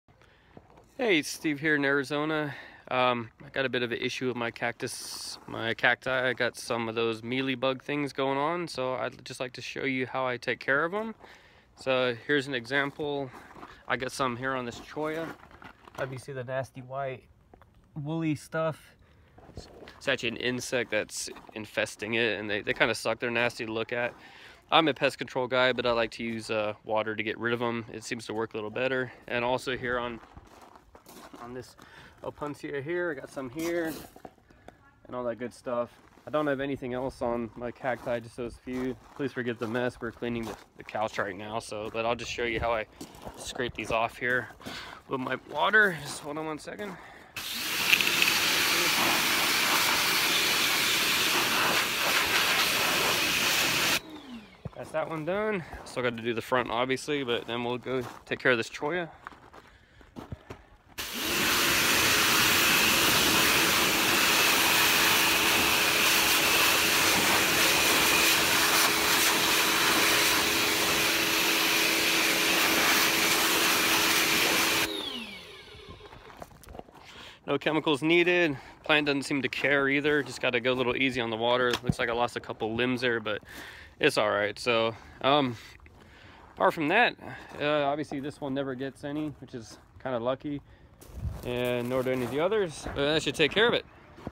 Removing cochineal bugs (mealybugs) from cacti
Forgive my tongue sticking out in the beginning I suck at videos. This is how I remove these pests from my beloved cacti without using chemicals.